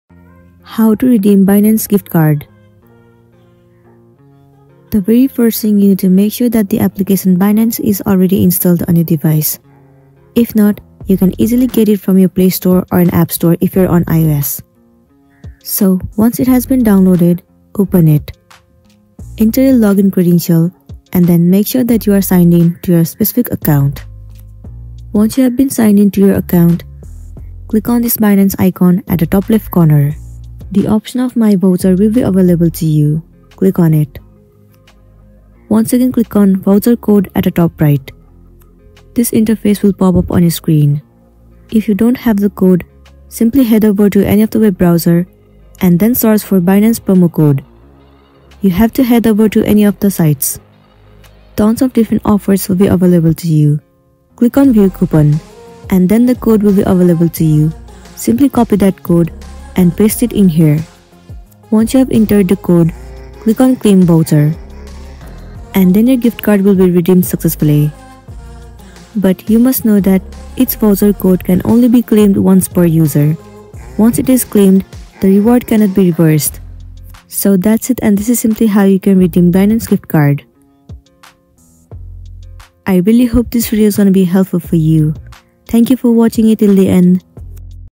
How to Redeem Binance Gift Cards 2024 (iOS/Android)
Redeeming Binance gift cards is a straightforward process that allows you to add funds to your Binance account conveniently. Follow this full guide for 2024 to learn how to redeem Binance gift cards hassle-free.

In this guide:
0:00 - Introduction
0:05 - How to Redeem Binance Gift Cards (2024) Full Guide

Discover the step-by-step process of redeeming Binance gift cards. The guide covers logging in to your Binance account or creating a new account if you haven't already. Learn how to navigate to the "Gift Cards" or "Redeem" section within your Binance account and enter the gift card code. Follow the prompts to redeem the gift card amount, which will be credited to your Binance account balance. Additionally, the guide provides tips on checking your account balance, using the redeemed funds for trading or other purposes, and ensuring the security of your account.

Enjoy the convenience of redeeming Binance gift cards with this comprehensive guide!